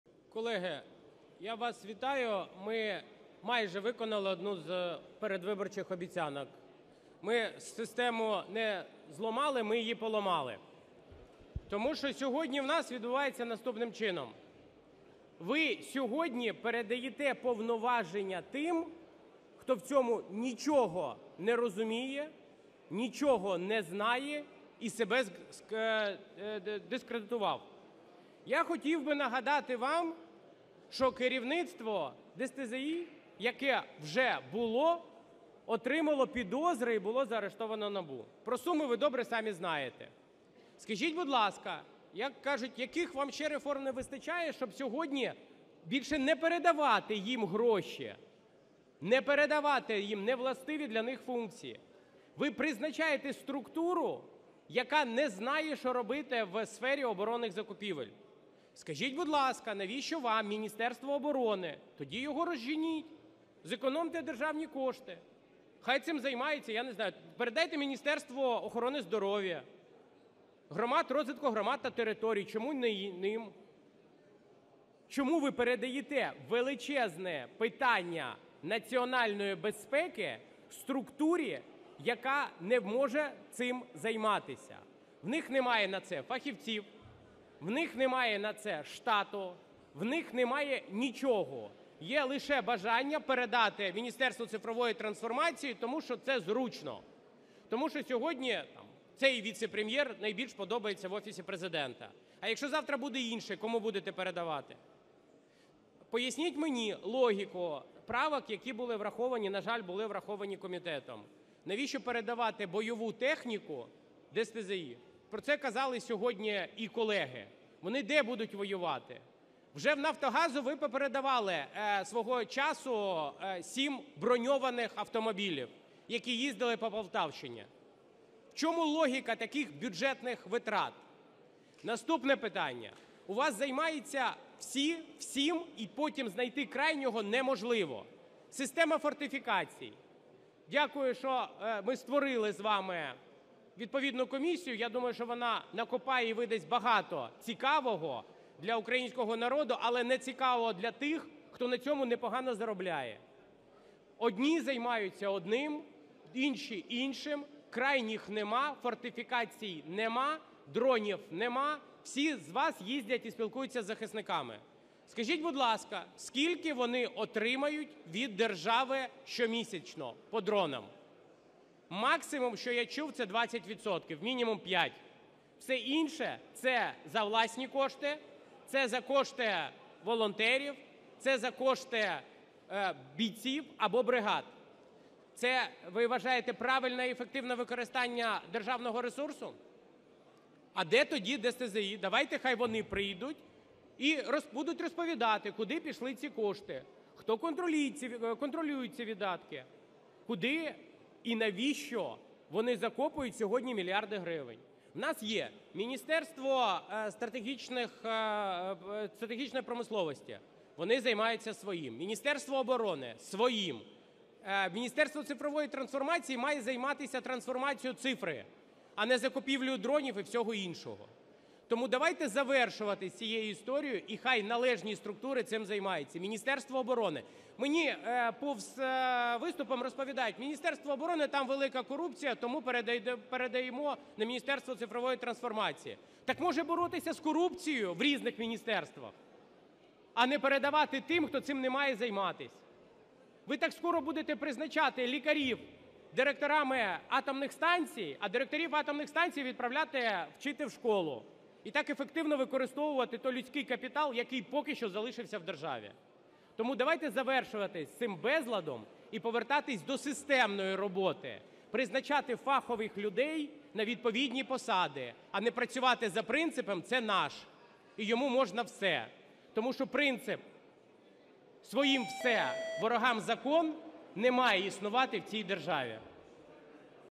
Разумков: Монобільшість продовжує руйнувати систему держуправління. Своїм все, а ворогам закон!
Монобільшість майже виконала одну з передвиборчих обіцянок. Вона не зламала систему, а поламала її!
Наприклад, вчора протягнули закон, яким передали ряд повноважень для Держспецзв’язку щодо забезпечення армії дронами, засобами РЕБ та іншою технікою і боєприпасами. Це абсолютно невластиві функції для цього відомства, яке ще й себе дискредитувало! Нагадаю, що минуле керівництво ДССЗЗІ було затримано за підозрою в розкраданні державних коштів!
Але влада продовжує виділяти цьому відомству гроші і пов’язувати з оборонними закупівлями, в яких воно нічого не розуміє! Причина – бажання передати ці повноваження Міністерству цифрової трансформації, якому Держспецзв’язку підпорядковується. Бо профільний Віцепрем’єр сьогодні найбільше подобається Офісу президента.
З осені минулого року Держспецв’язку отримує 45% від «військового» ПДФО з громад на дрони. Хто спілкується із захисниками знають, що вони отримують від 5% до 20% дронів від держави щомісяця. Все інше – куплене за кошти волонтерів, або власні кошти бійців чи бригад!
Кожне міністерство повинно займатися своєю справою. От і Мінцифри має займатися цифровою трансформацією, а не закупівлею дронів! Для цього в нас є Міноборони. А розповіді, які я чую про те, що «там велика корупція», то краще передати ці повноваження Мінцифри», не витримують жодної критики!
То може варто боротися з корупцією?! Час завершувати з цим безладом і повертатись до системної роботи, призначати фахових людей на відповідні посади, а не працювати за принципом «це наш і йому можна все»! Бо принцип «своїм все, а ворогам закон» не має існувати в нашій державі!

Приєднуйтесь, аби мати ще більше актуальної і важливої інформації!